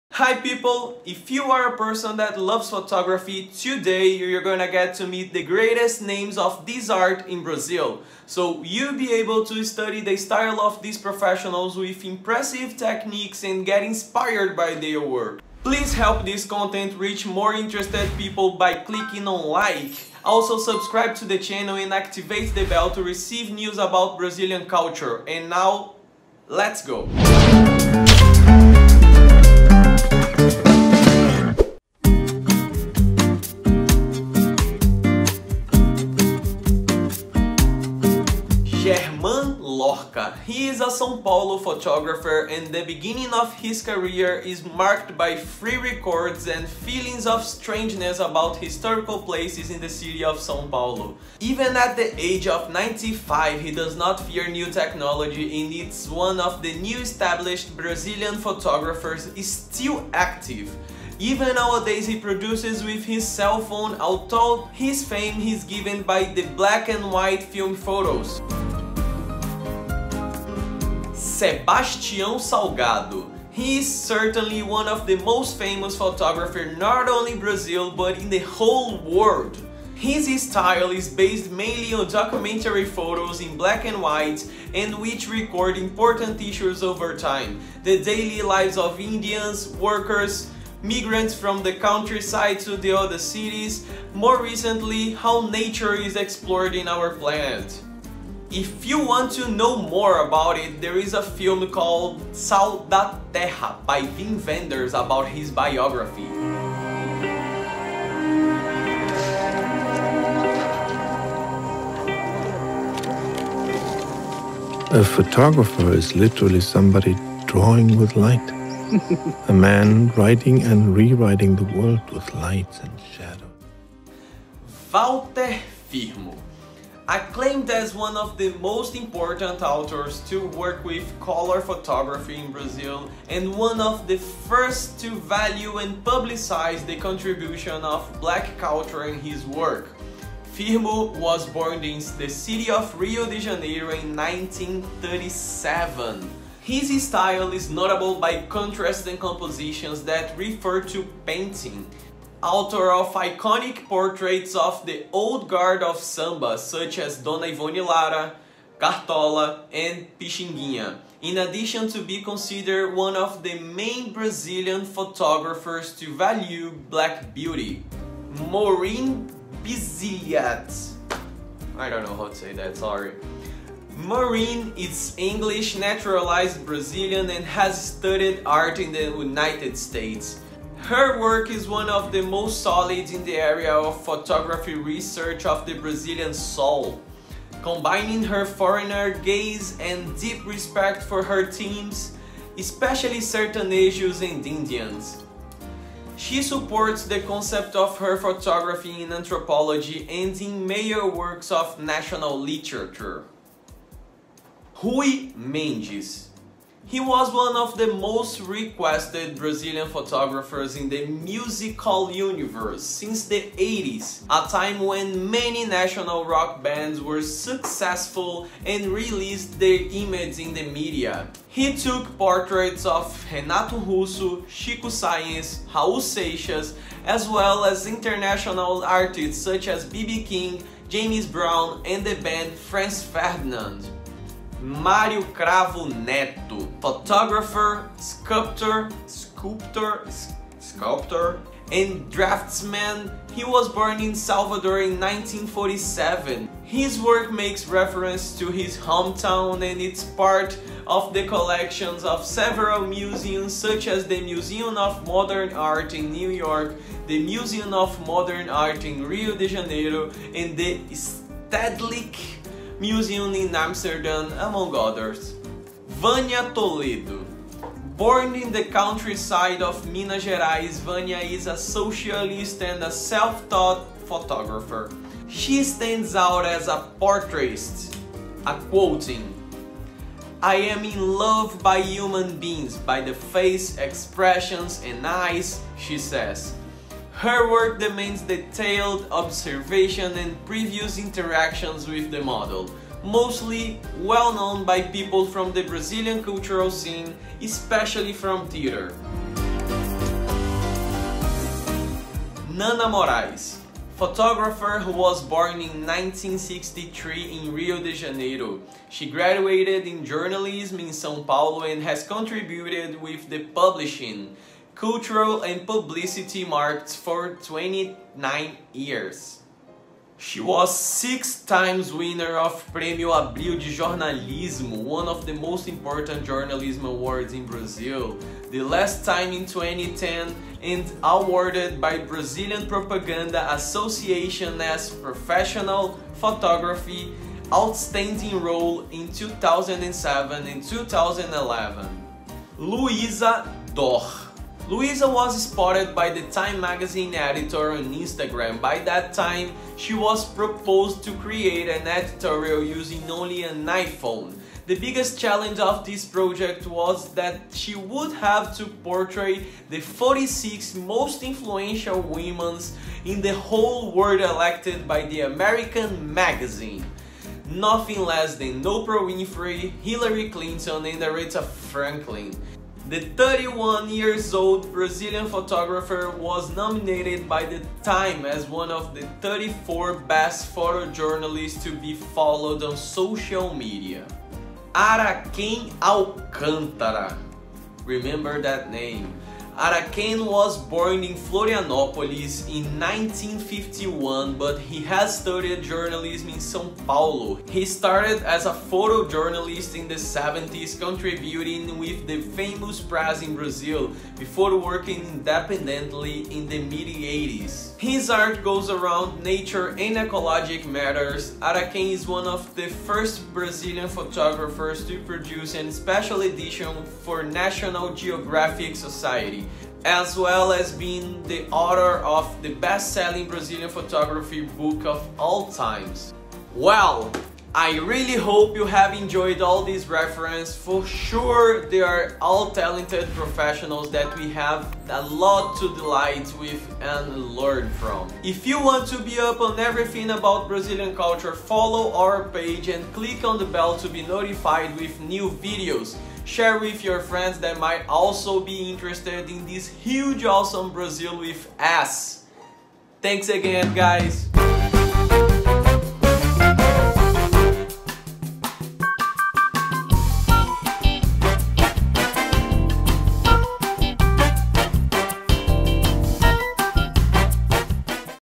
BRAZILIAN GREATEST PHOTOGRAPHERS - 10 references to learn from
Oi gringos!

If you are a person who loves photography, today you're gonna get to meet the great names of this art in Brazil. You will be able to study the style of professionals with impressive techniques and get inspired by their work.

I really hope you have enjoyed all these references. For sure, they are all talented professionals that we have a lot to delight with and learn from.

Please help this content reach more interested people by clicking on like.

Follow our page and click on the bell to be notified with new videos!

Facebook, Instagram and Twitter: @brwiths